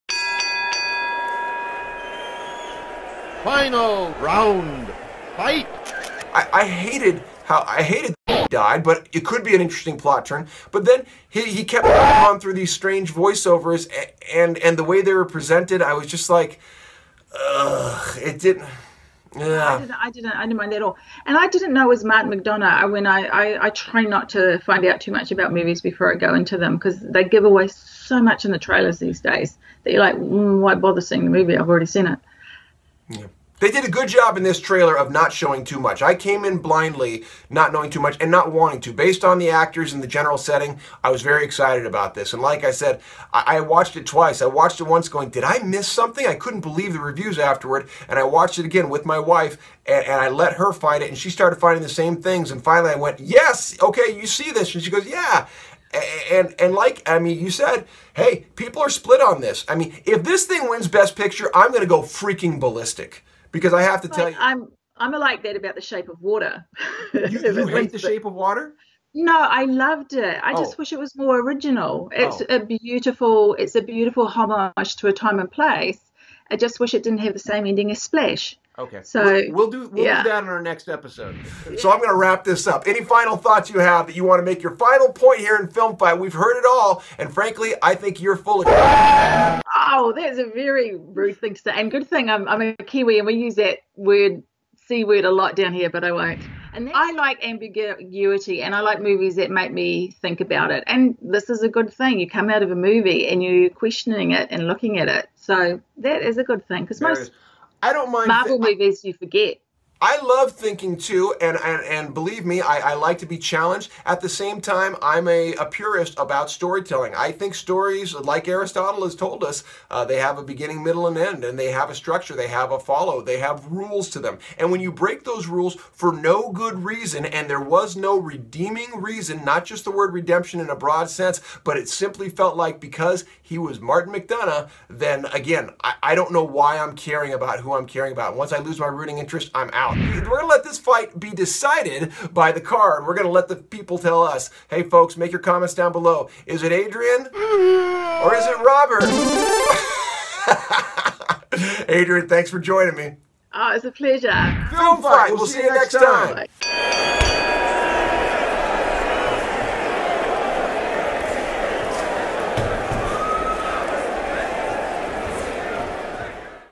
Film Fight - Three Billboards - Final Round!
In Round three, Robert says the Quentin Tarantino void has left audiences looking for replacements like Martin McDonagh, while Adrienne continues her case for the validity of damnation. Now it's on in the Final Round! Tell us who you think won or give your own opinion below. What film do you want to fight for?